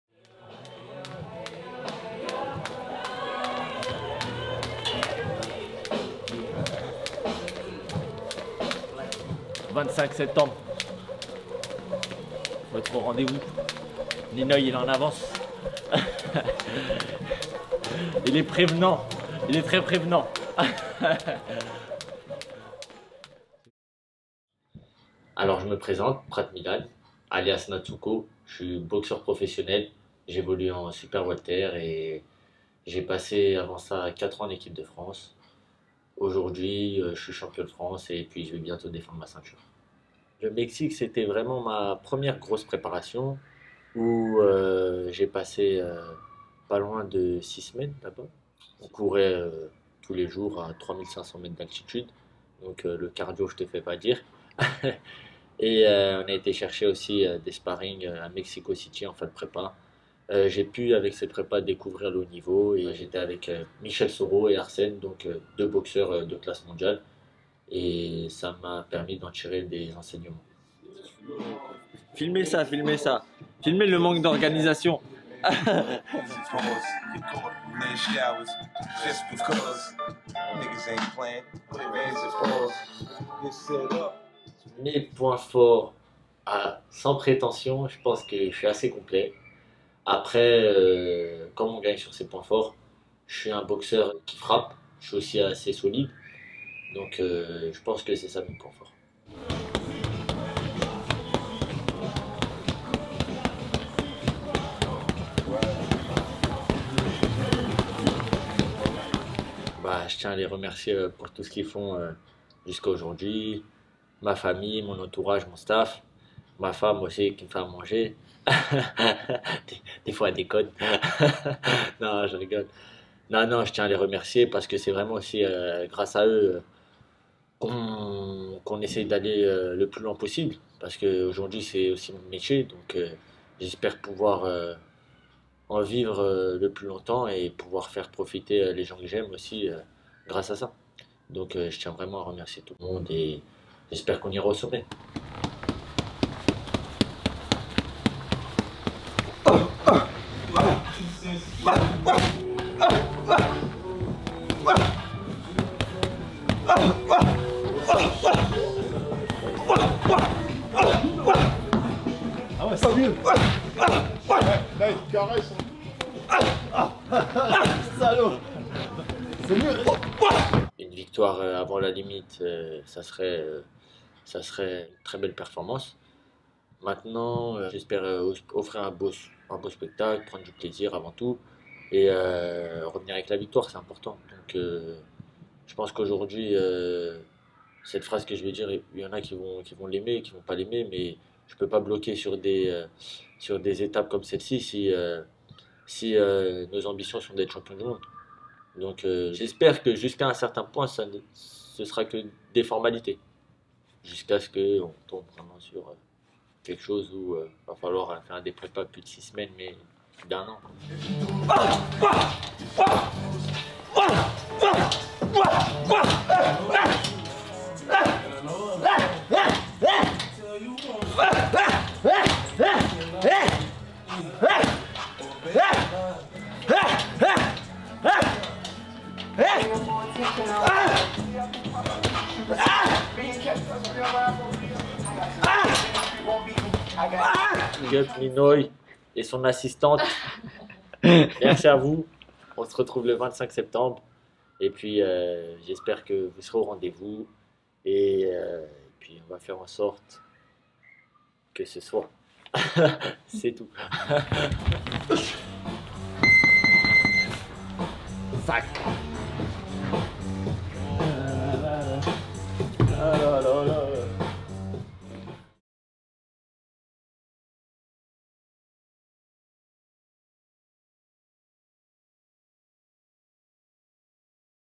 Portrait de boxeur - Milan Prat
Immersion dans la préparation de Milan Prat, alias Natsuko, au club de Drancy.
Il défendra sa ceinture de Champion de France des Super-Welters le 25 Septembre 2021.